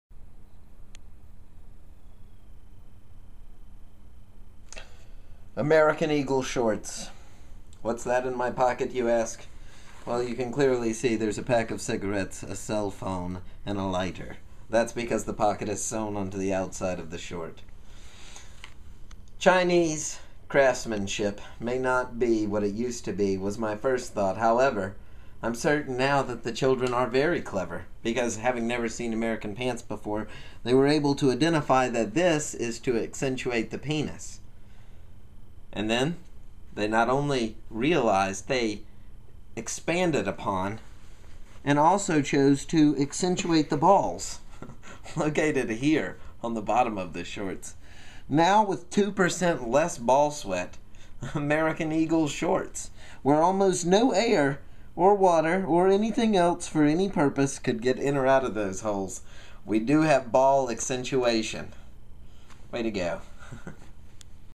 Ball Shorts
Shorts that take it the extra mile, balls in, the best shorts to buy. Well maybe not, but an interesting conversation piece never the less.